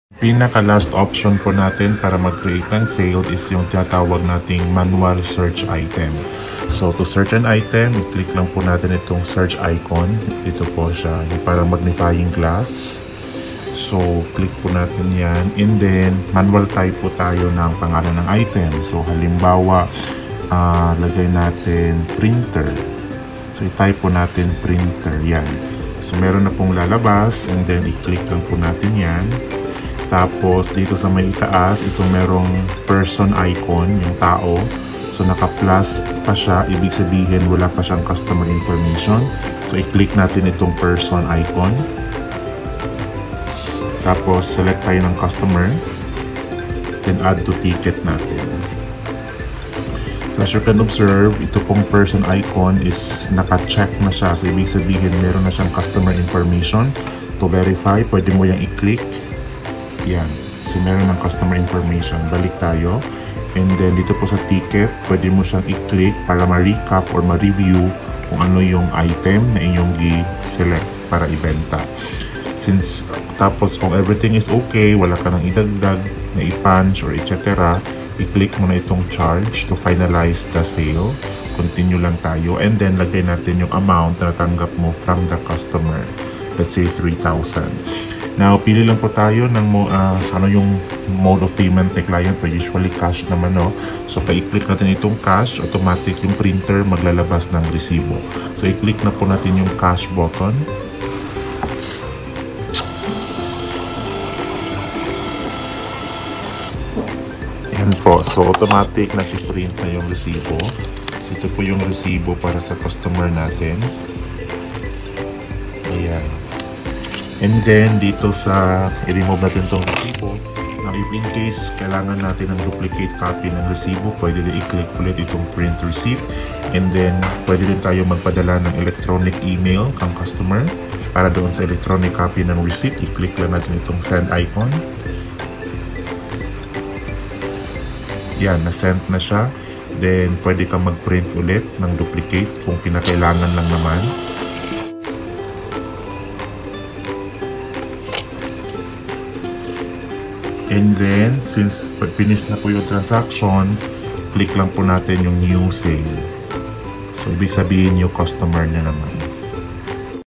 How To Create a Sale using Search Button
How To Create a Sale using Search Button (option 3)

Mobile POS & Customer Loyalty System
- pwede offline
- pwede online
- compatible on android cellphone and tablet
- compatible with thermal receipt printer